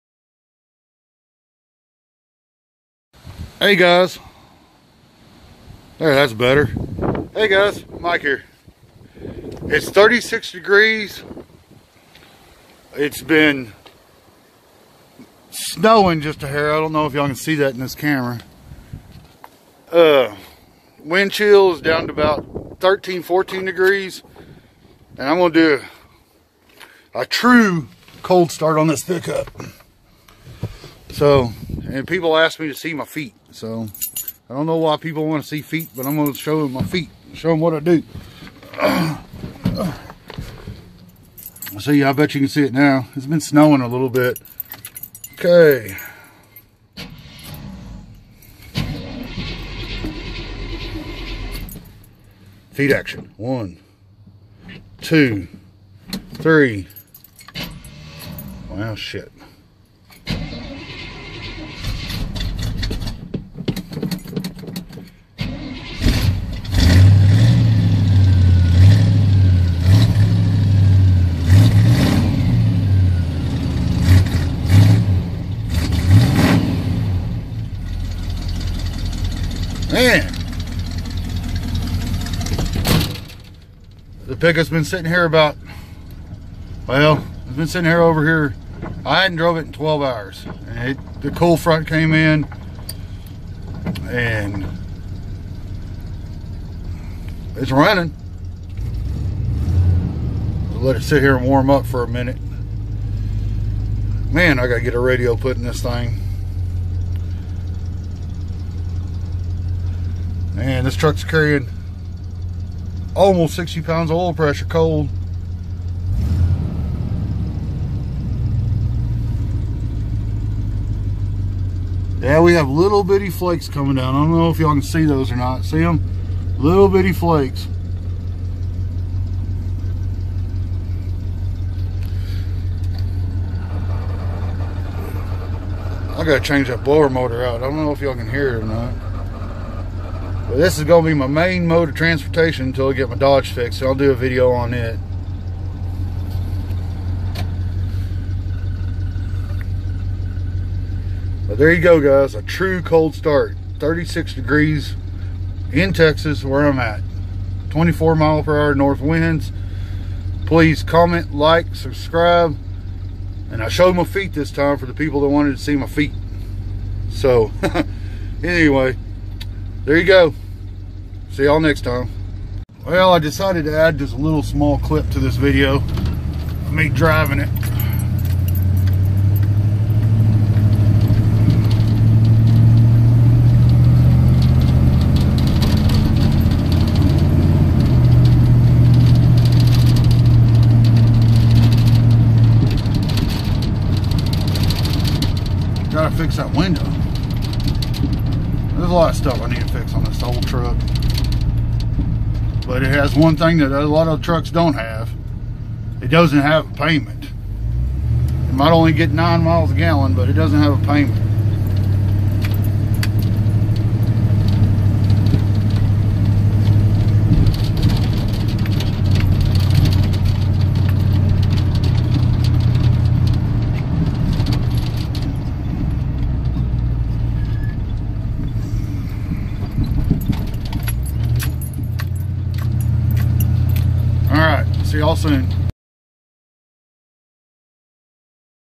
Cold start in Texas. 35 degrees in Texas
Farm truck cold start.